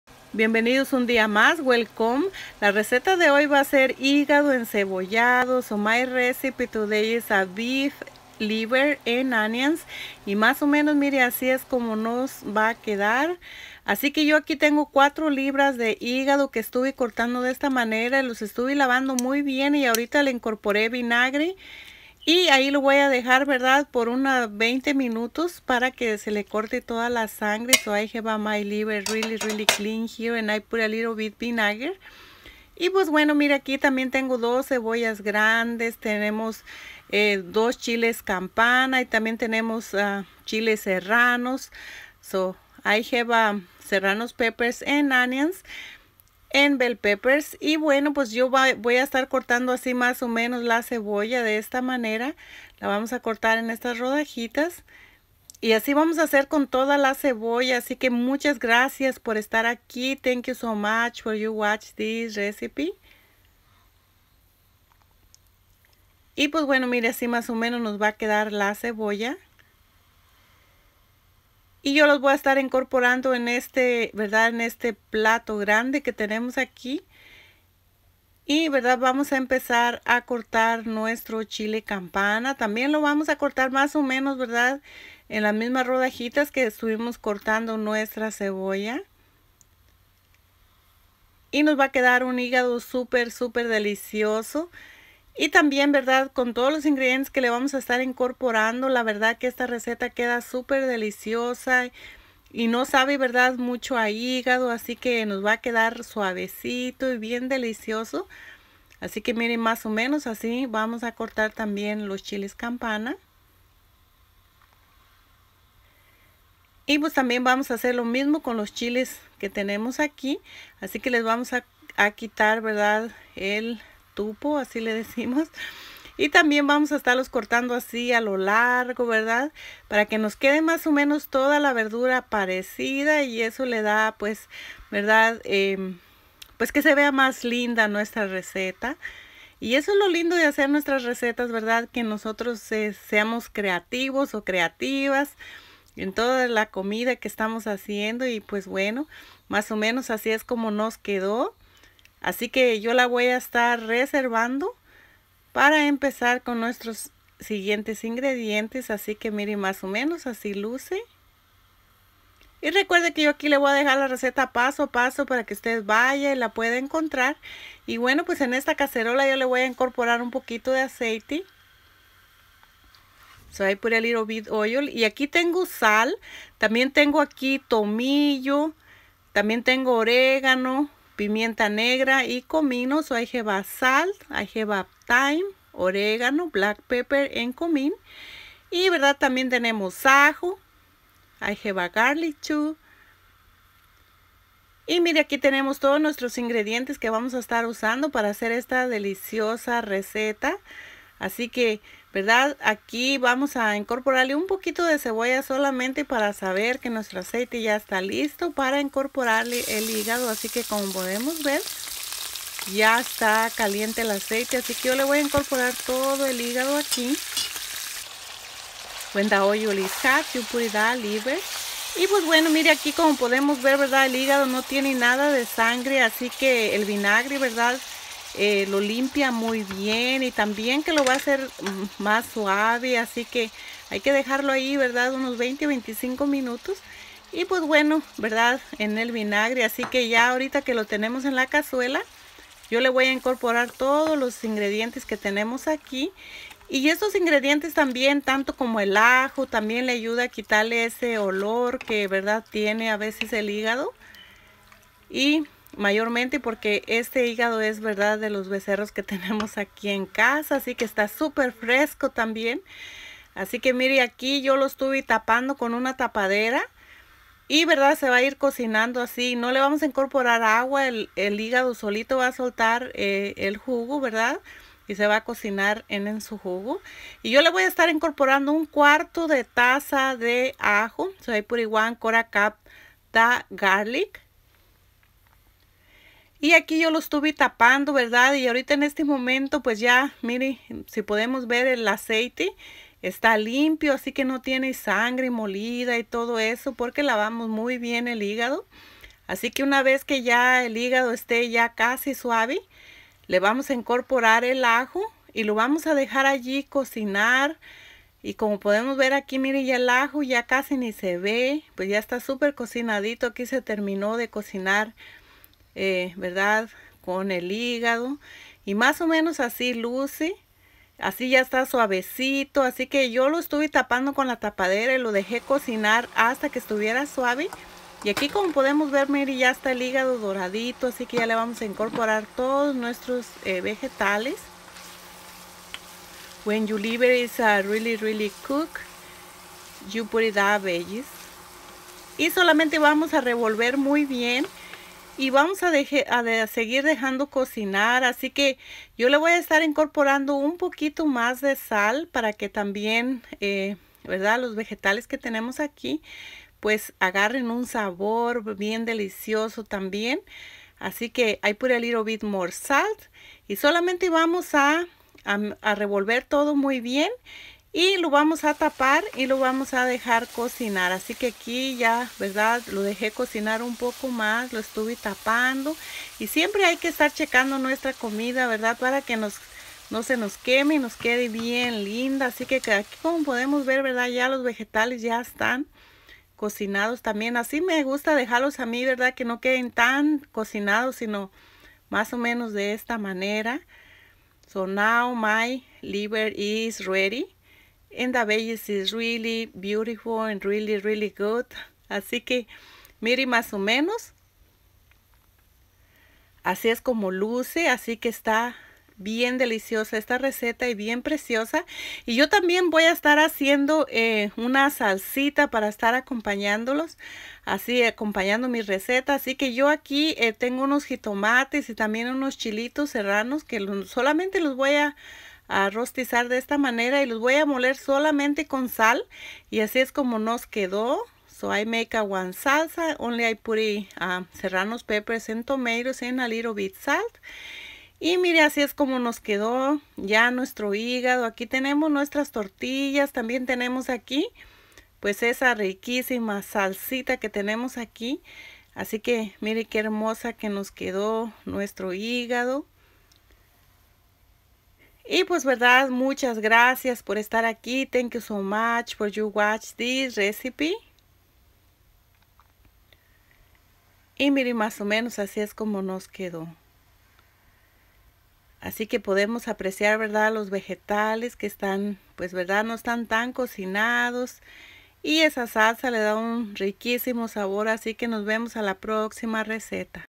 Cómo hacer igado encebollado suavecito y sin olor 🙏🏻👍🏼❤️how to make beef liver
4 lb de igado de res
2 cebollas grandes
2 chiles campana
4 chiles serranos
1 cucharada de pimienta negra
1 cucharada de orégano 1 cucharada de tomillo 2 cucharadas de sal
Aseite al gusto
1/4 de tasa de ajo fresco picado

Salsa jitomates y chiles ceranos asados
Sal al gusto
4 lb of beef liver
2 bell pepper 2 onions.  1/4 garlic cup
4 ceranos pepper
1 tablespoons of oregano
1 tablespoons of cumin
1 tablespoons of tyime
2 tablespoons of salt
Salsa tomatoes ceranos peppers roasted
Salt need